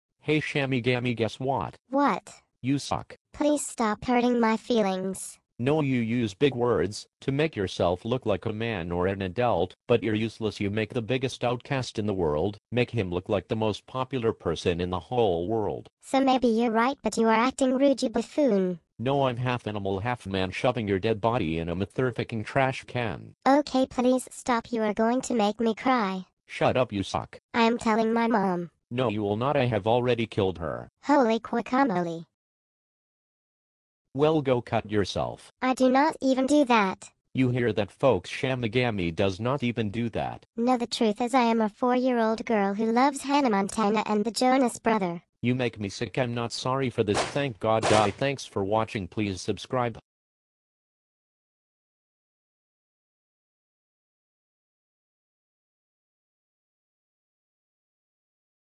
SIVIgames vs. shammigammi round 1
here is the first round of me aka SIVIgames versus that coward shammigammi